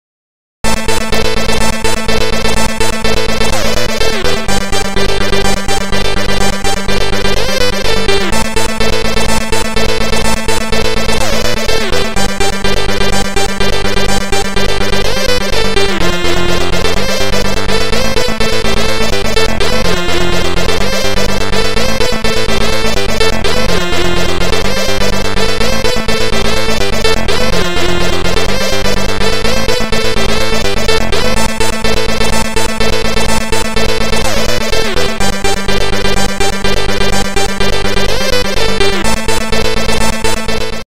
Tristar & Red Sector Inc. - Hetero Intro #12 - ht-trs12
for Tristar & Red Sector Inc.
in 1998 January 20
coder Hetero
artist Double Trouble
musician Emax